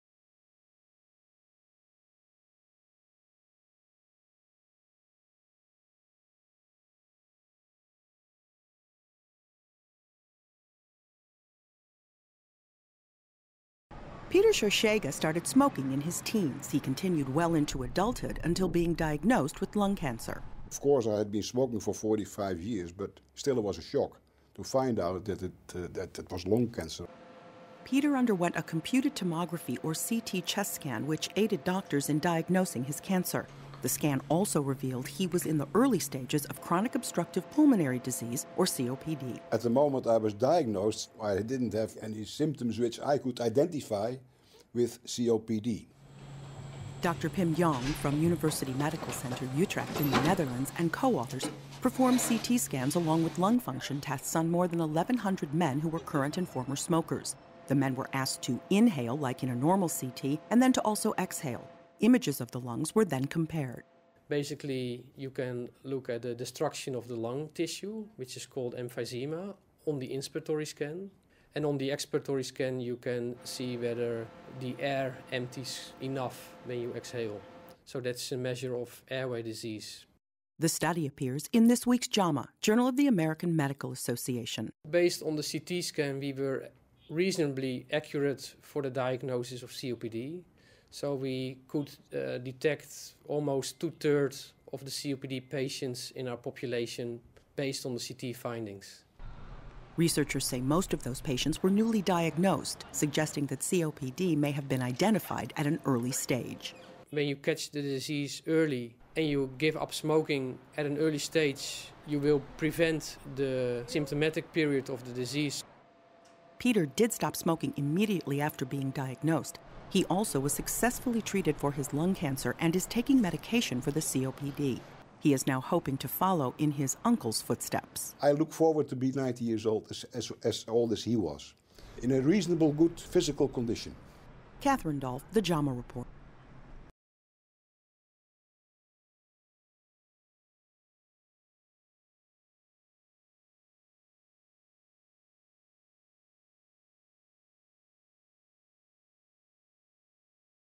2011-10/26 LUNG CANCER C-T SCANS MAY IDENTIFY SMOKERS W/CHRONIC OBSTRUCTIVE PULMONARY DISEASE
Chronic obstructive pulmonary disease or C-O-P-D is a major cause of death for heavy smokers. The onset of the disease can be insidious and is often underdiagnosed until it reaches a more advanced stage. A new study finds using computed tomography or C-T scans when screening smokers for lung cancer can also help detect C-O-P-D. Catherine Dolf has more in this week's JAMA Report.